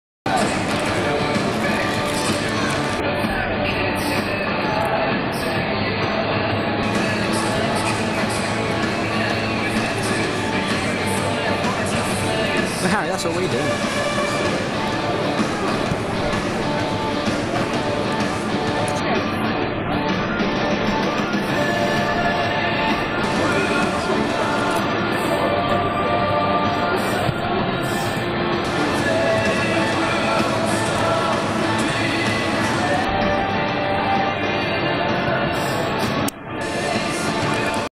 West Ham vs Chelsea 2013 - Chelsea Players Warm up
A short clip of Mark Schwarzer and the rest of the Chelsea squad before the big London derby against West Ham and Chelsea at The Boleyn Ground.
Row A in The Sir Trevor Brooking stand, great seats right at the front!